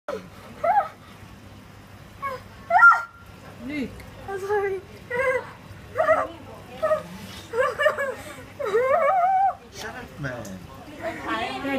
Feet sucking Fish - hold tight Luke prt 2
And not doing very well. These fish are damn ticklish.